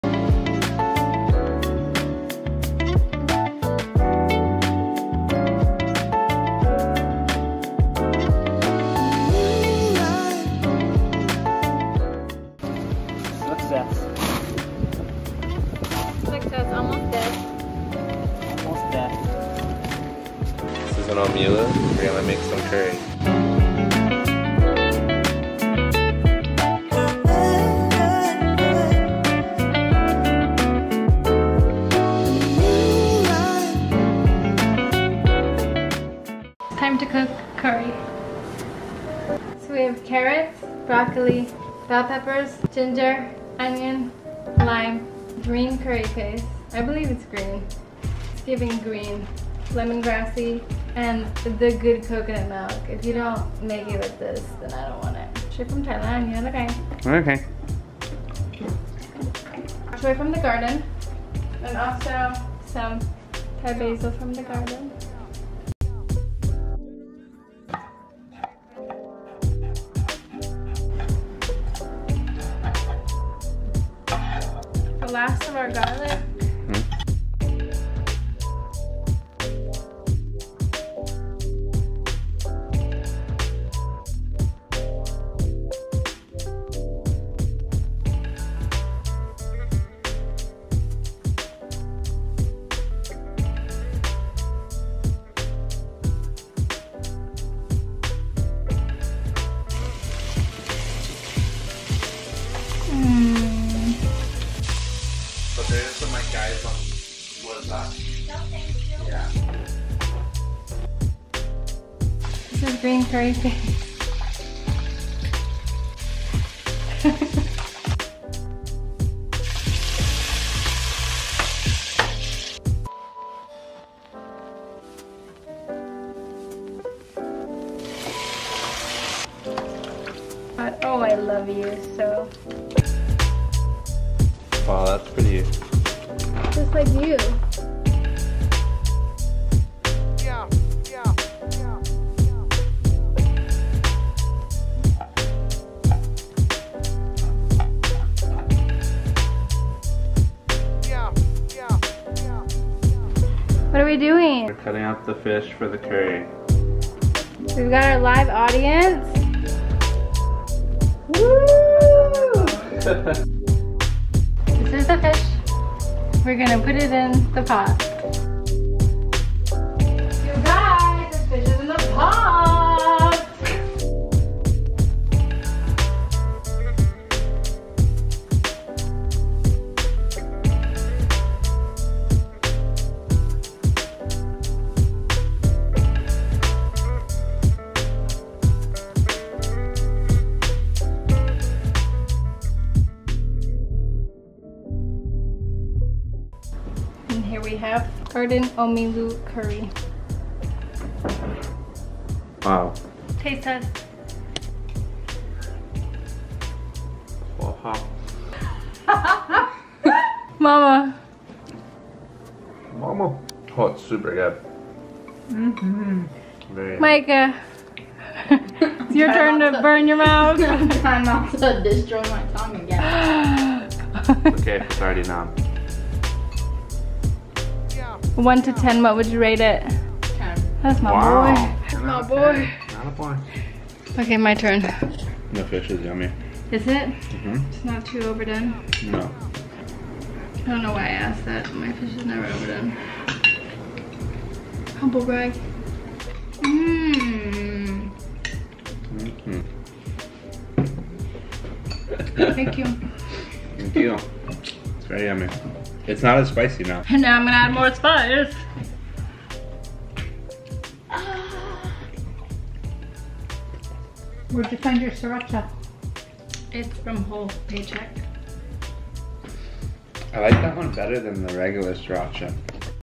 FISH CURRY CATCH AND COOK
Omilu (travelly) curry with herbs and veg from the garden made in 15 min! Fresh, nutritious, and delicious.